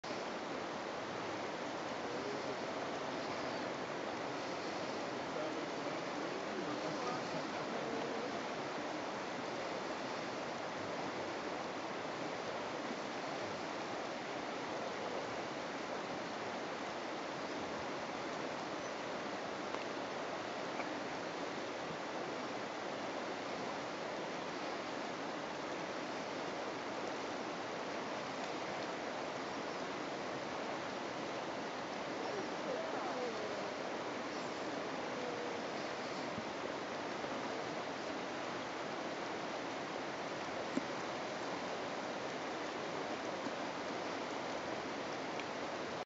Black Bear by Rogue River
A black bear walks along the Rogue River